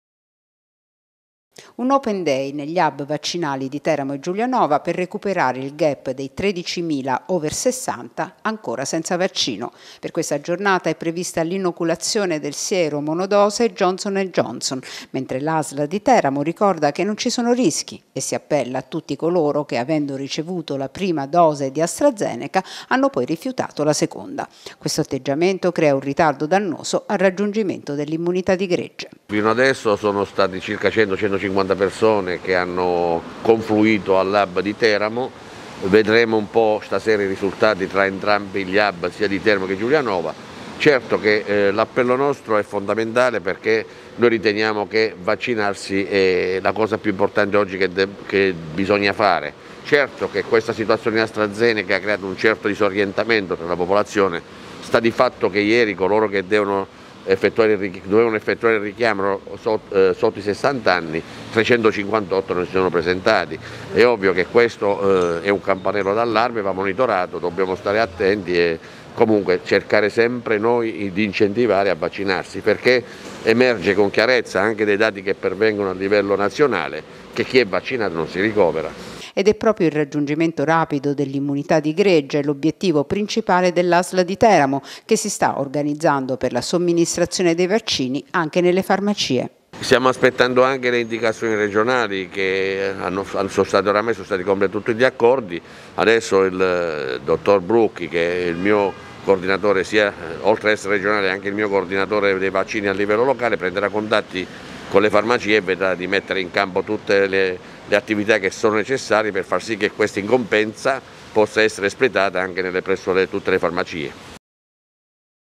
Teramo. Open day per 13 mila over 60 ancora senza vaccino
L'Asl di Teramo ha indetto per la giornata odierna un open day negli hub vaccinali del capoluogo e di Giulianova dedicati agli over 60 che non hanno ancora ricevuto alcun vaccino. Si tratta di circa 13 mila cittadini ai quali sarà somministrato il siero monodose Johnson&Johnson.